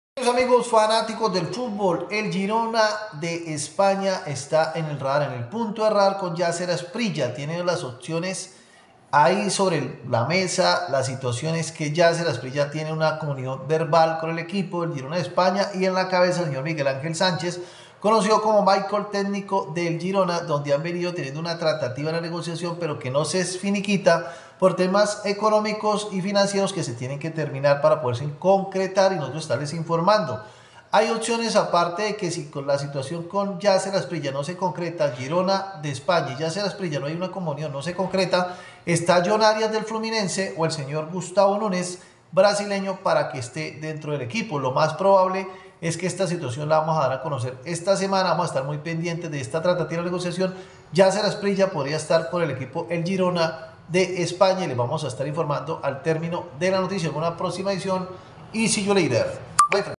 jugador colombiano en la mira de equipos de primera división
este jugador esta en la mira de varios equipos y ya es hora de llegar a primera division